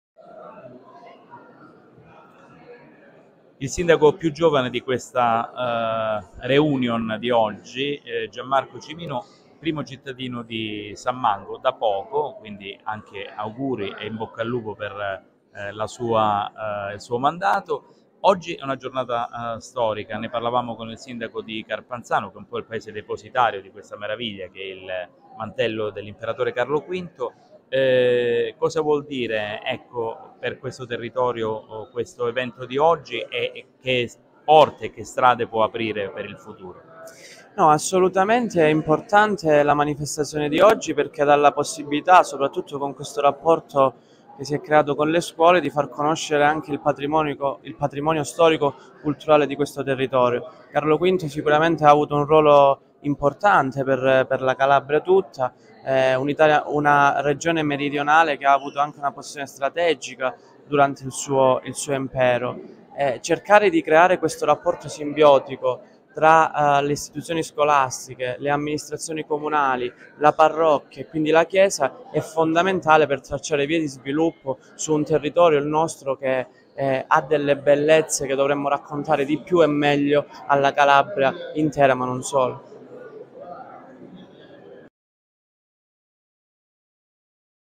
Carlo V il sovrano spagnolo in Calabria Museo d'arte sacra ''Paolo VI'' di Martirano
Intervista al sindaco di San Mango d'Aquino, Gianmarco Cimino, in occasione dell'incontro "Carlo V: il sovrano spagnolo in Calabria". Museo d'arte sacra ''Paolo VI'' di Martirano.